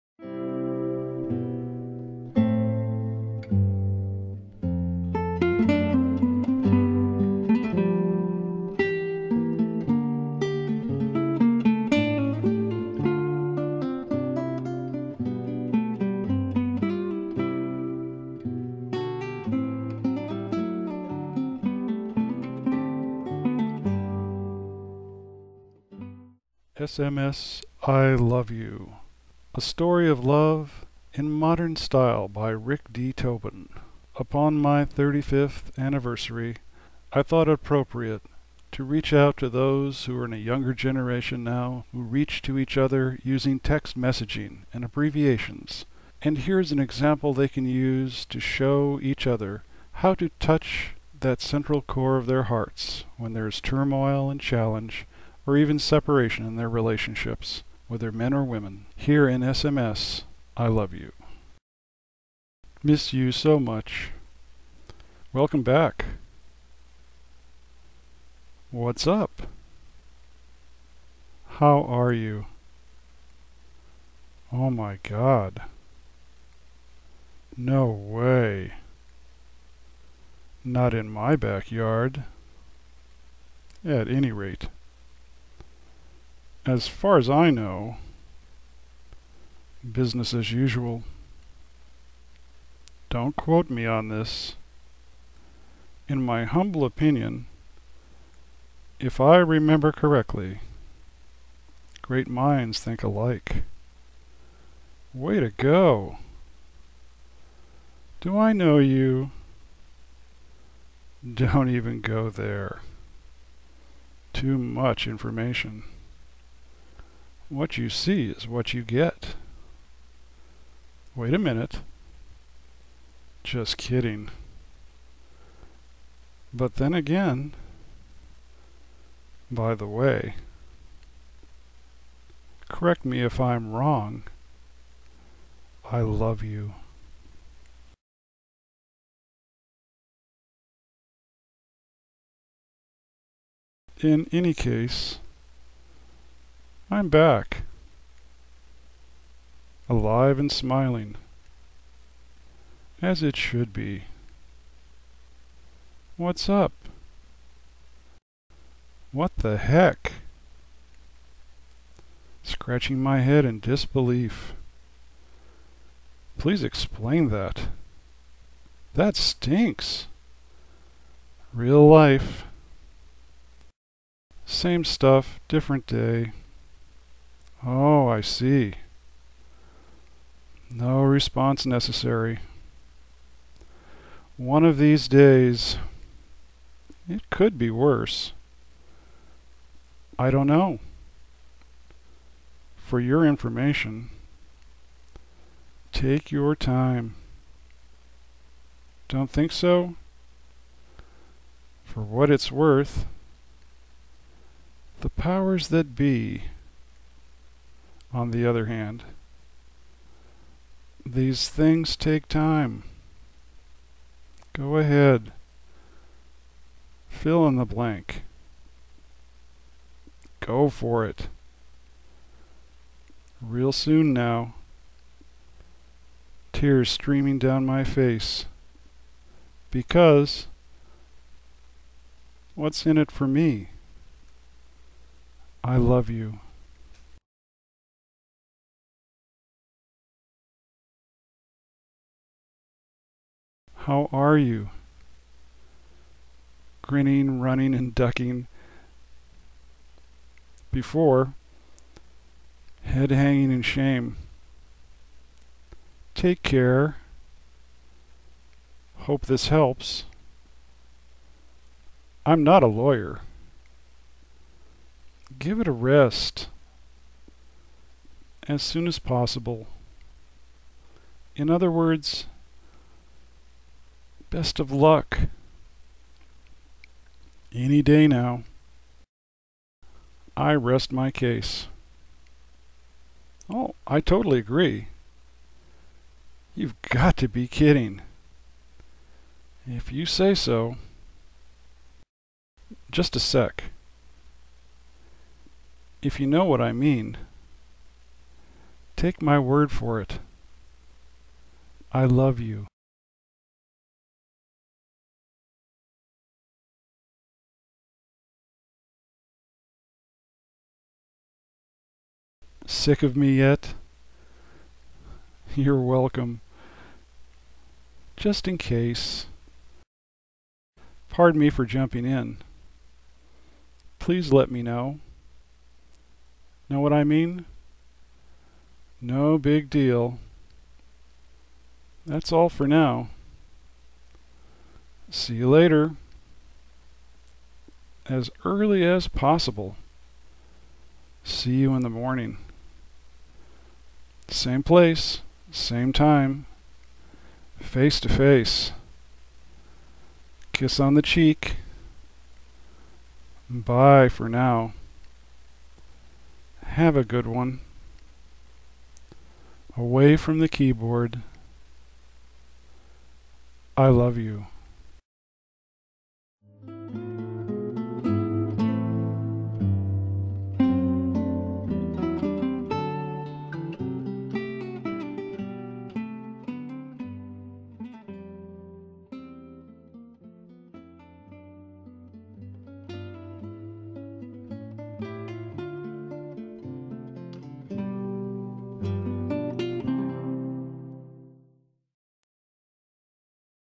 SMS ILU
The original thought behind this video was to produce a clever conversation between two lovers using all the strange and wacky abbreviations used to text ideas with acronyms without the effort of fully writing out ideas.

This is a video I produced ten years ago, on my 35th wedding anniversary, as a means to honor my love for my wife Cinda. Today, July 7, 2022, is my 45th anniversary. SMS ILU was created a year before the strokes started taking her away from me, in pieces, starting in 2013. I'm loading this again, today, in honor of our decades together as we struggle through her later stages of hospice care.